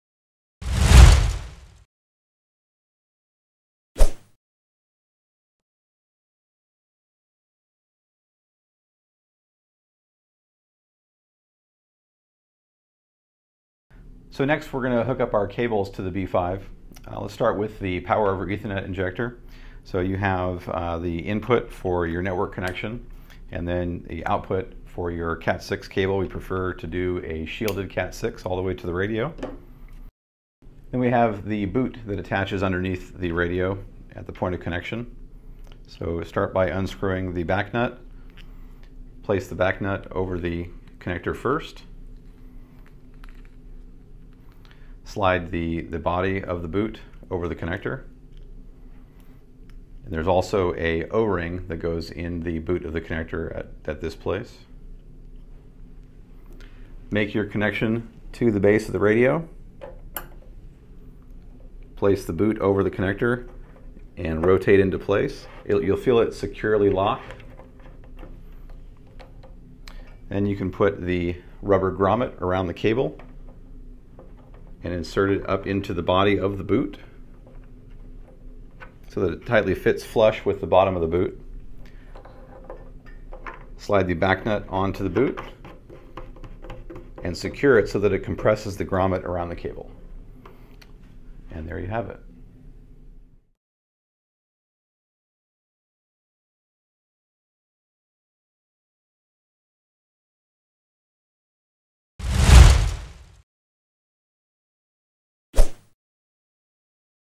Install POE & Gland Installation for the Mimosa B5 (Backhaul)
A quick demonstration of the Installation steps of the POE and Gland for the Mimosa B5 Backhaul.